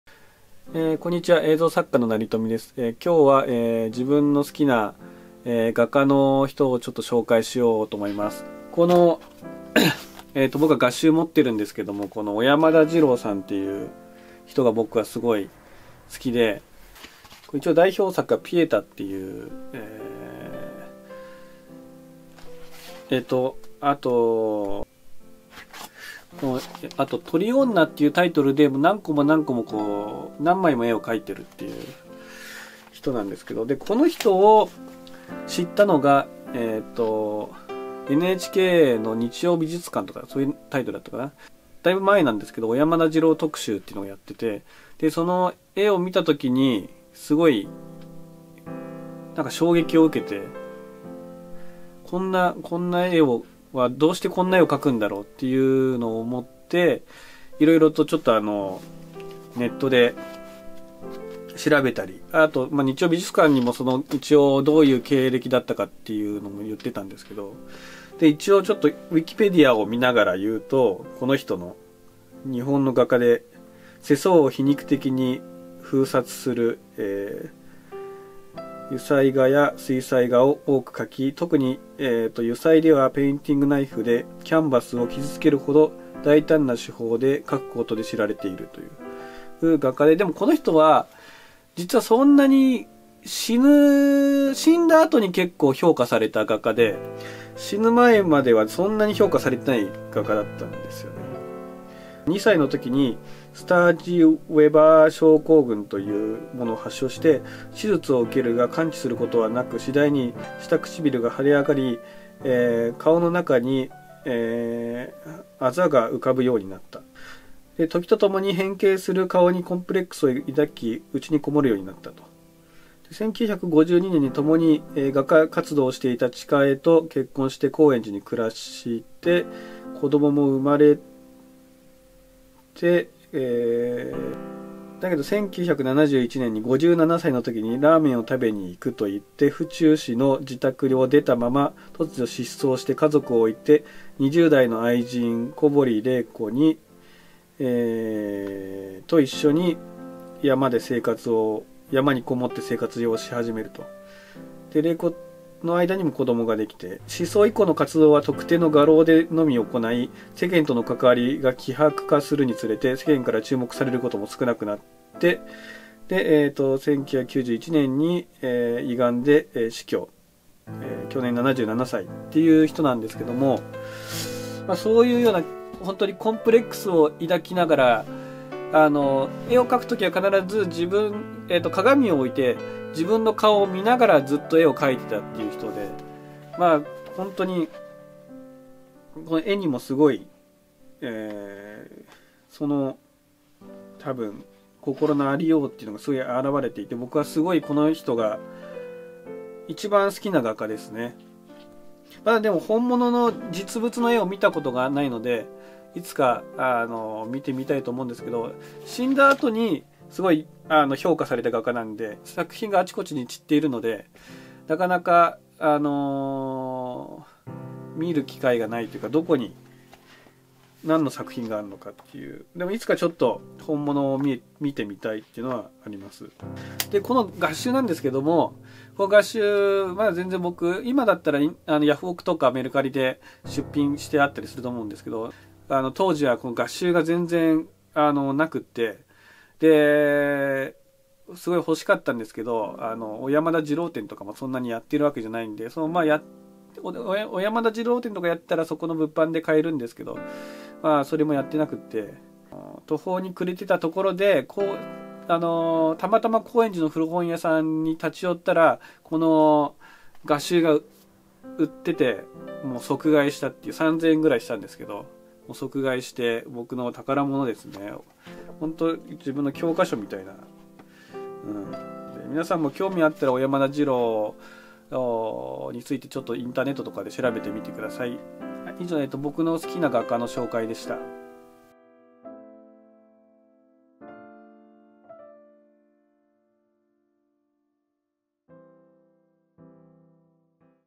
57歳で家族を捨て山に失踪した画家 小山田二郎
生きていたらドキュメンタリーが撮りたかったですね

Twitter(DM解放しております）